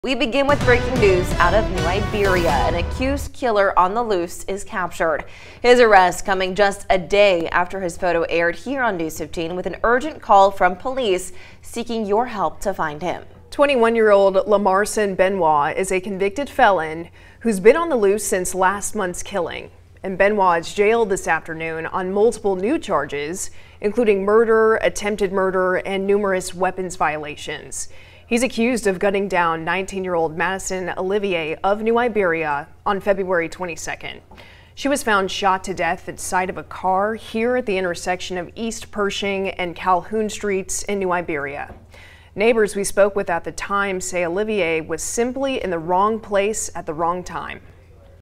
Suspect arrested for murder of 19-year-old New Iberia woman
The New Iberia Police Department reported the suspect wanted for the shooting that claimed the life of a woman in February has been taken into custody.